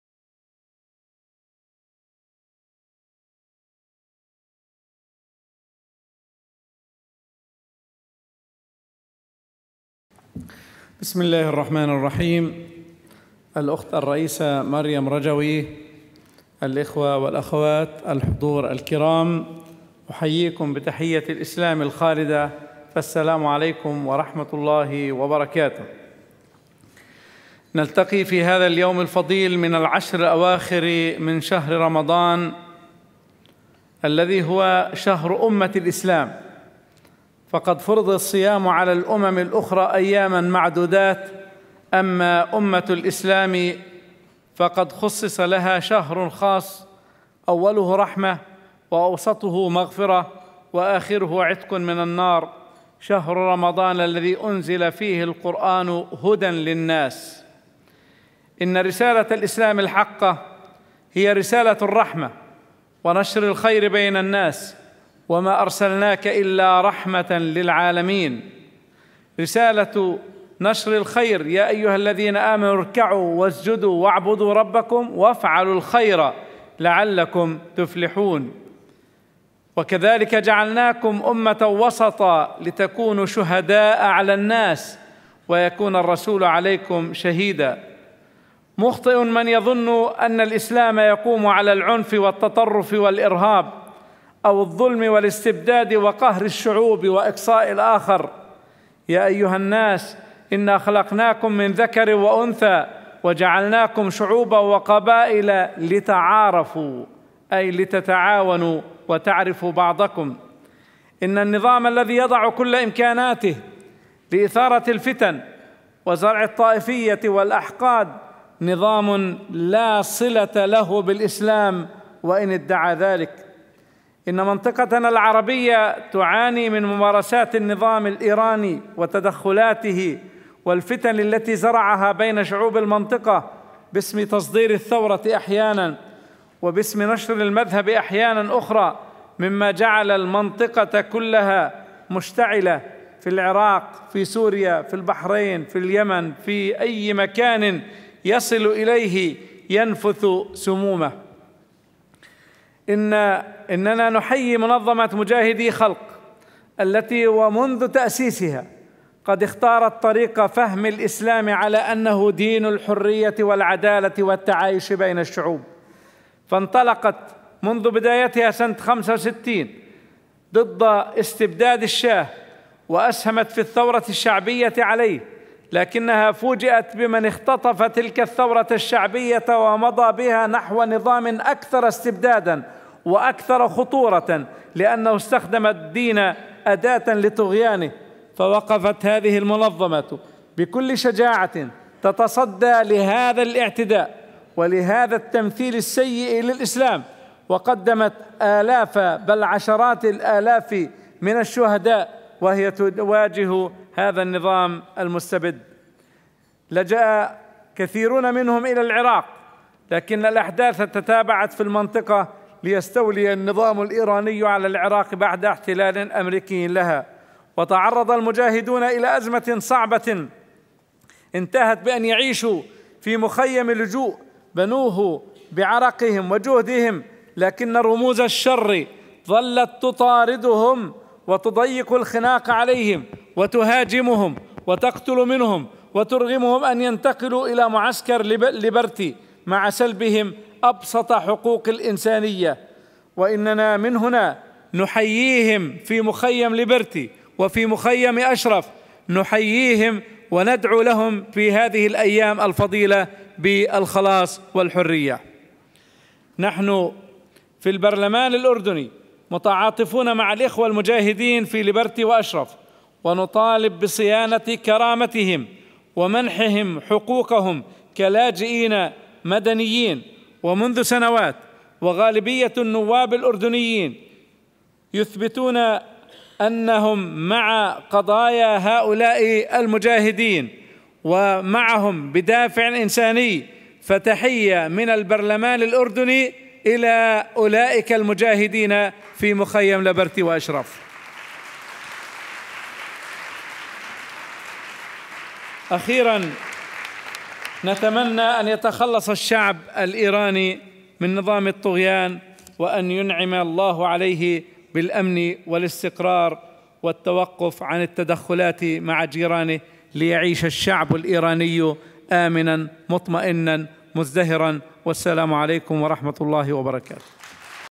Mohammad Al Haj Jordan MP Ramadan Ashraf Liberty True Islam
On the occasion of Month of Ramadan
United against misrepresentation of Islam
"Islam for freedom, peace and brotherhood, against war, repression and fundamentalism."
Saturday, August 3, 2013 - Paris

On Saturday, August 3rd, on the occasion of sacred month of Ramadan, representatives from Arab and Islamic countries along with representatives from Islamic communities in European countries warned against growing meddling of mullahs' regime in countries in the region. They also underscored the need for solidarity against this regime that is the leading state sponsor of terrorism and main promoter of fundamentalism and religious dictatorship in today's world and the most important factor in sowing discord and warmongering among Muslim.